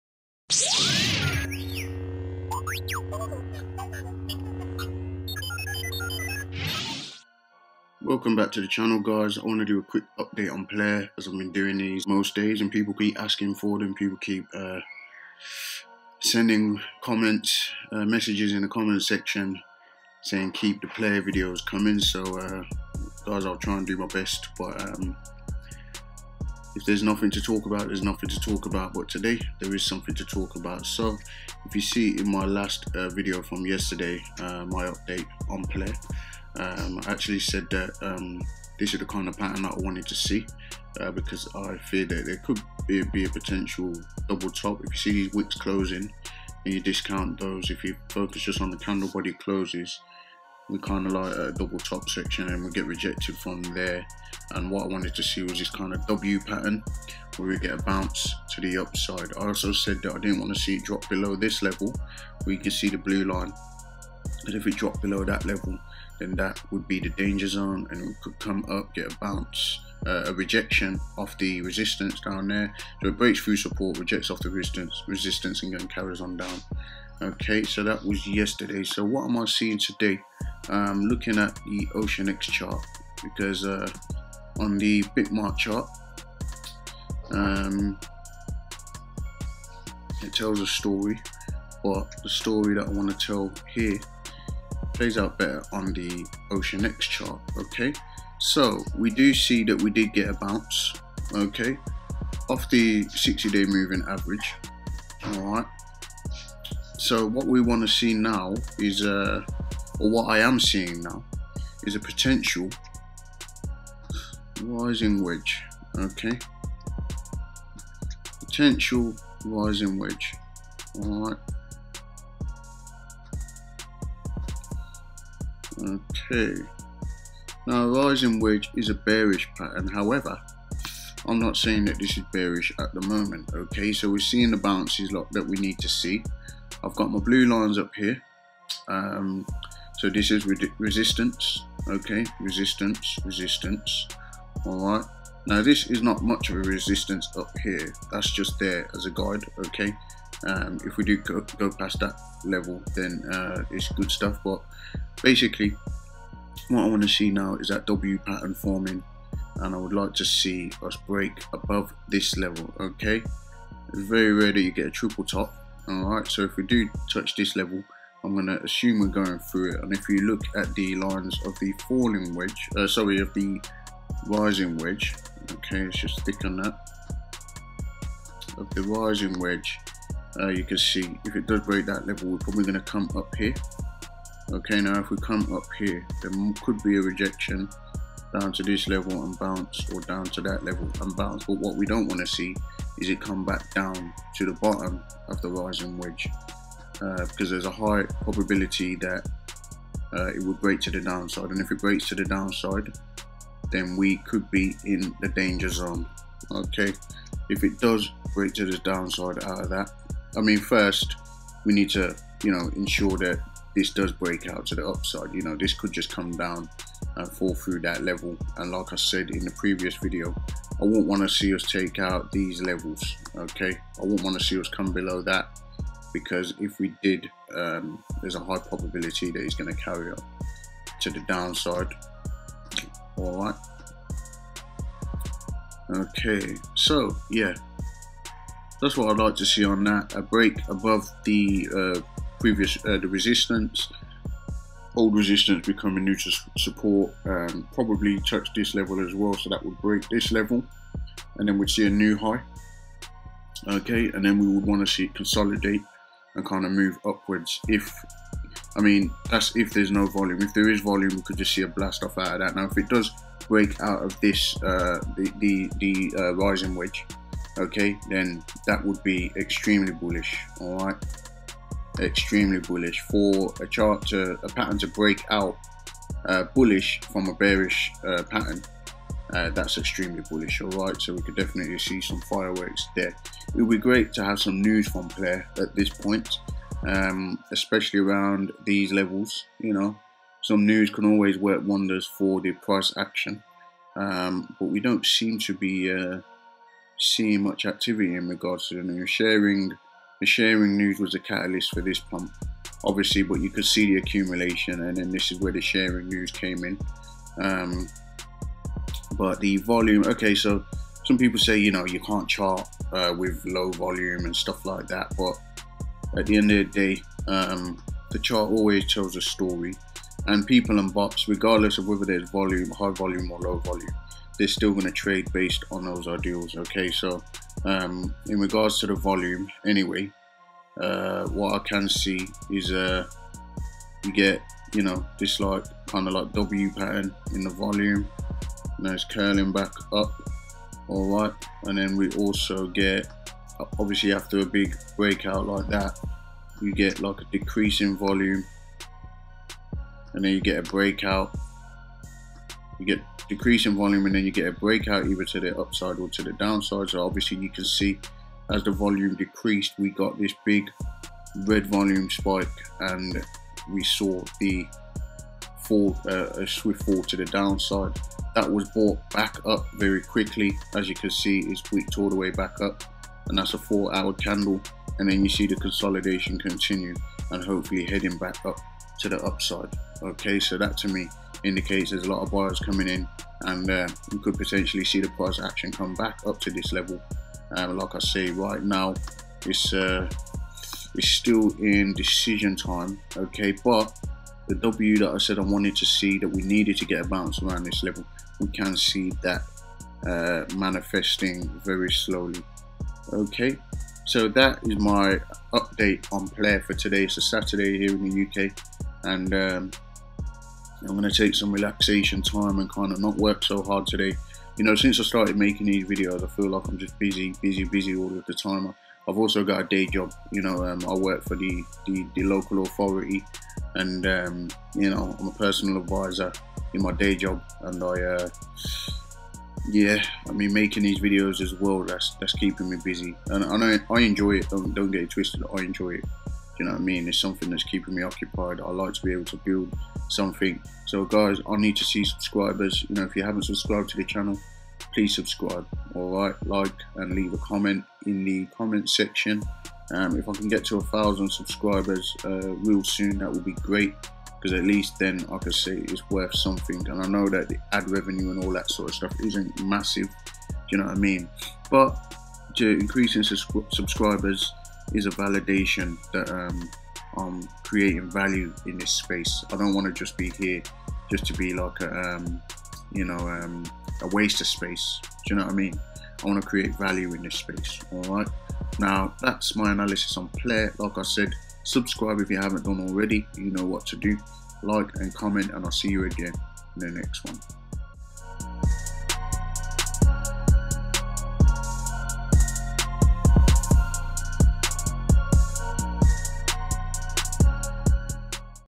Plair PLA Bouncing | Upside Potential | HODL Analysis Update
Plair (PLA) is holding this level as we ride this weekend. Where will it go from here? Here is my short term forecast and technical analysis on todays price action using the OceanEx and Bitmart exchange charts. I believe there is more upside potential but we need to see some key levels hold. Unless you are a Ride or Die HODLER!

I do not expect them, but donations are always accepted and appreciated:
My addresses -
BTC: 3A9EcsoaZQquiu4uVGQojXbE84VHGrDnp8
ETH: 0x789315ae0c3f32ae6a4a38c01dac4054282fbf61